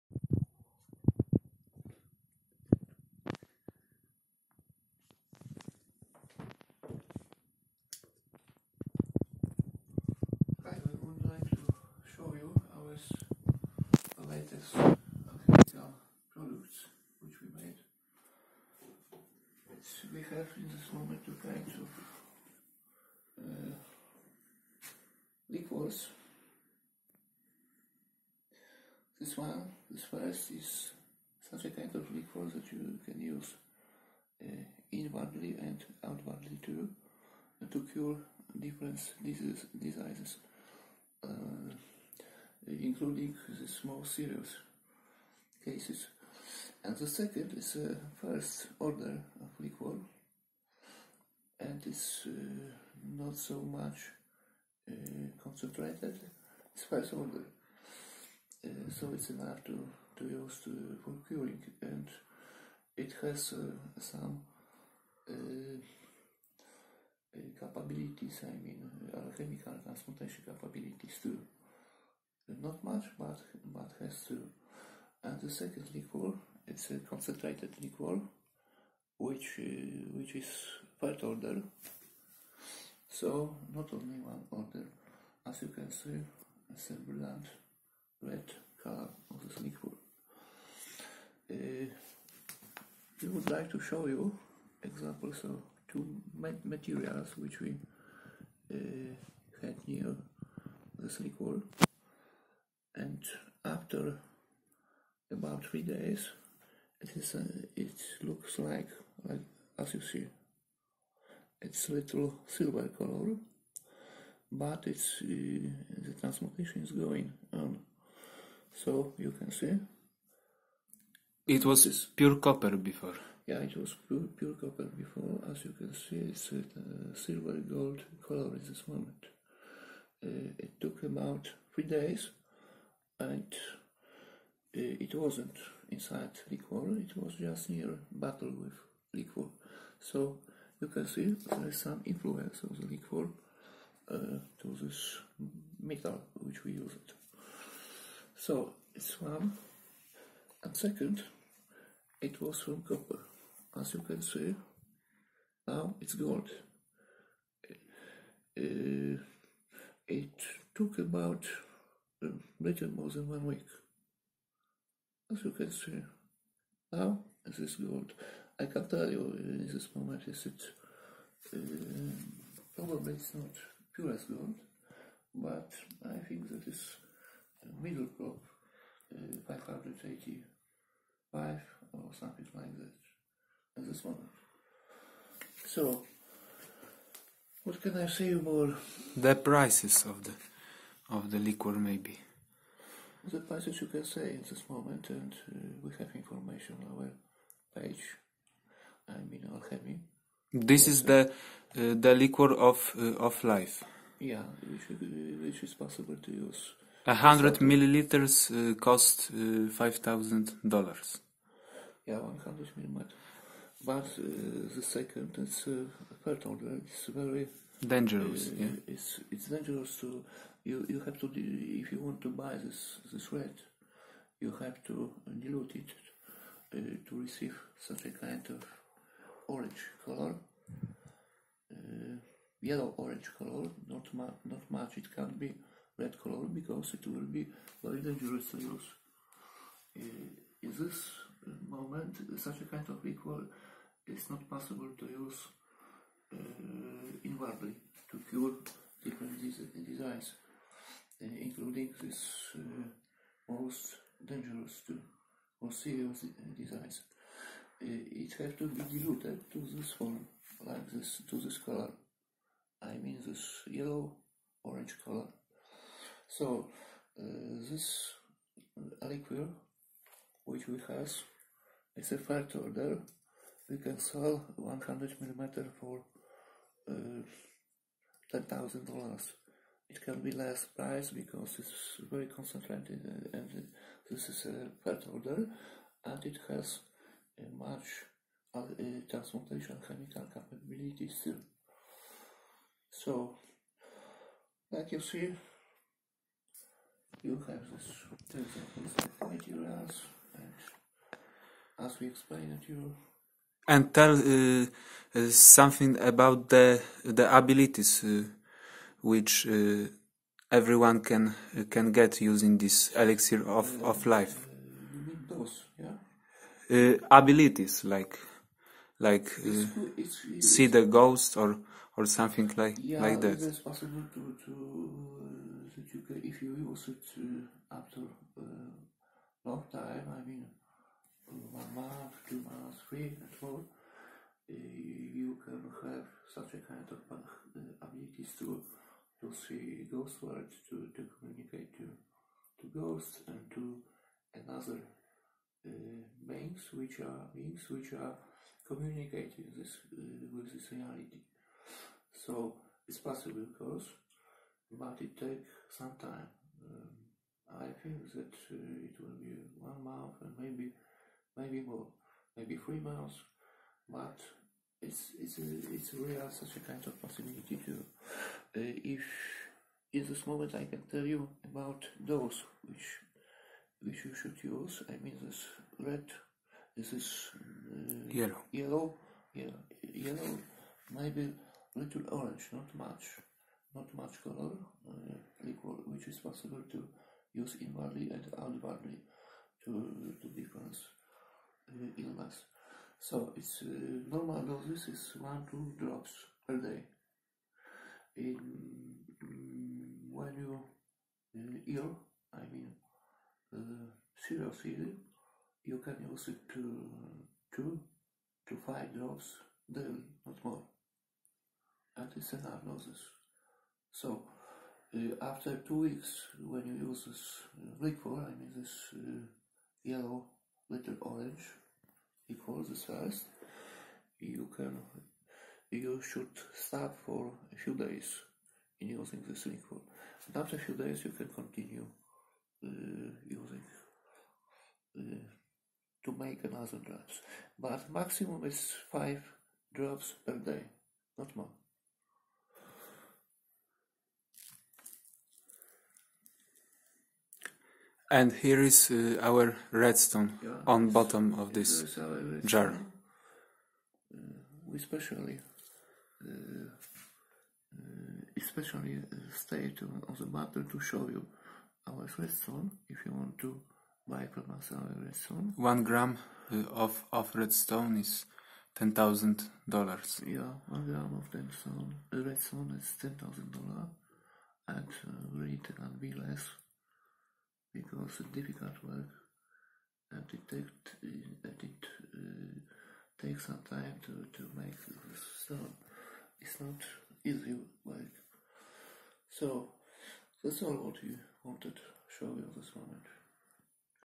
Real Alchemy Liquors from Red Stone (Gold)
We made 2 Liquors from Gold - (Red Stone of Gold)
Power of Sun is in the Liquors and the liquids have posibilities to transmutate metals and cure serious sickness. It can prolonge life if You use it long time.
Here You can contact with Us